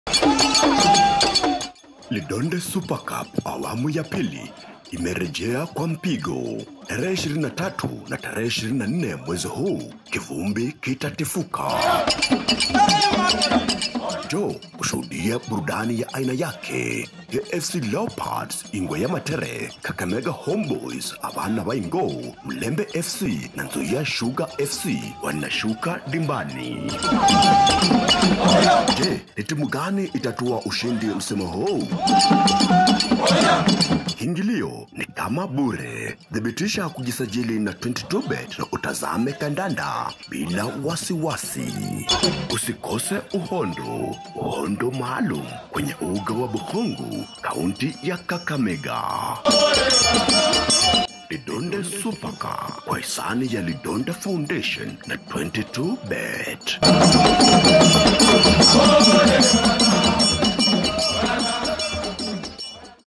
LIDONDE SUPER CUP KICK OFF ON 23TH-24TH DECEMBER AT BUKHUNGU KAKAMEGA COUNTY
Luhyiaisim🥰🥰.Bana Beru Khwakane Mubukhungu Mucounty ya Kakamega- Lidonde Super CUP.AFC Leopards SC Kakamega Homeboyz FC Nzoia Sugar Fc Mulembe United Football Club tsilaronya mushikuriii... 22BET Kenya BolaNaTetei BulalaAdvert. Done By Your Truly Certified Luhyia Son, Son Of The Soil Jesse Asutsa Tetei